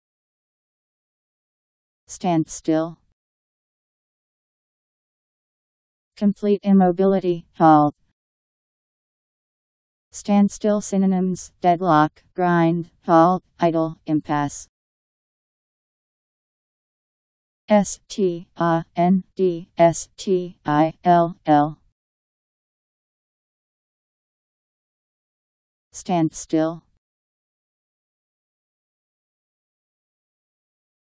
Standstill Meaning
Video shows what standstill means. complete immobility; halt. standstill synonyms: deadlock, grind, halt, idle, impasse.  standstill pronunciation. How to pronounce, definition by Wiktionary dictionary. standstill meaning. Powered by MaryTTS